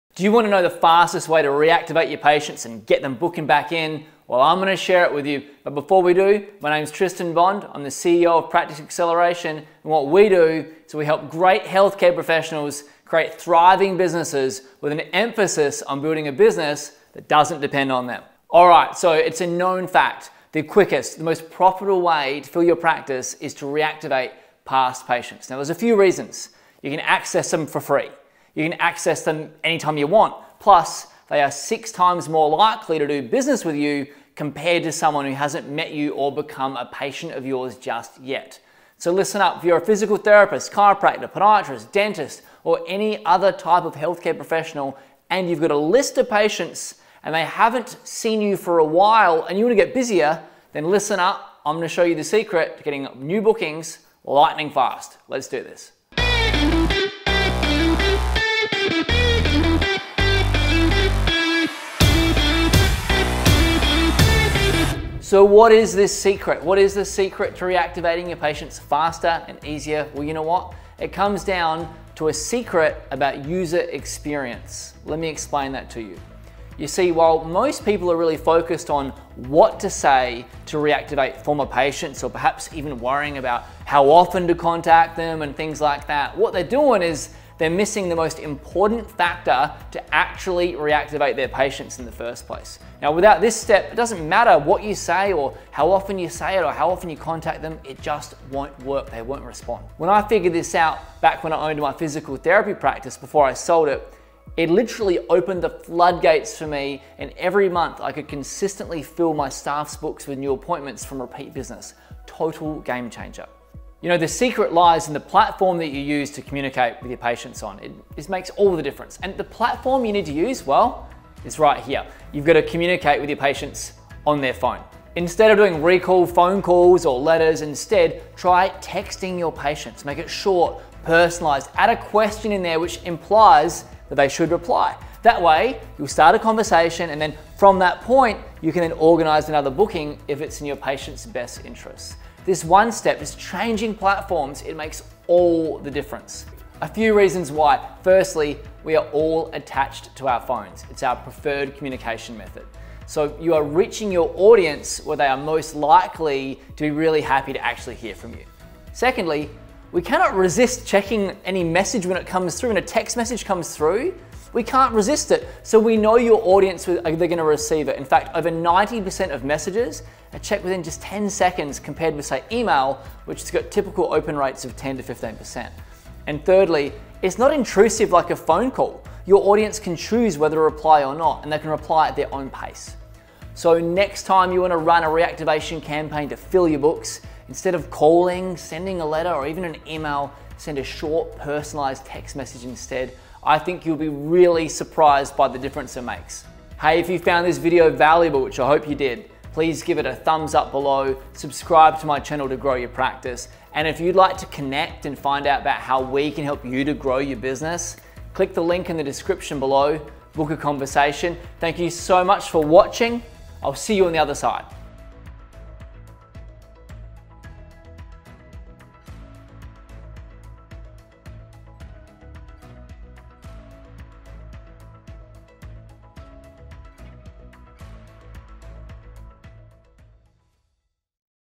The Fastest Way Of Communicating With A Patient To Reactivate Them
Do you know the fastest way of getting your inactive patients booked again?

In this video, I'll share with you the best ways of communicating with a patient you want to reactivate and win back.

It's a known fact that reactivating past patients is the quickest and most profitable way of filling your practice.

Why? Because you can access them for free and anytime you want. They are also six times more likely to do business with you compared to someone who hasn't met you or become a patient just yet.

But how can you achieve this? What's the secret approach to convince them faster and easier?

It's all about user experience and I'll explain everything you need to know about this and more.

Join me to learn how to master the process of communicating with a former patient and reactivating them to fill your practice quicker than ever before!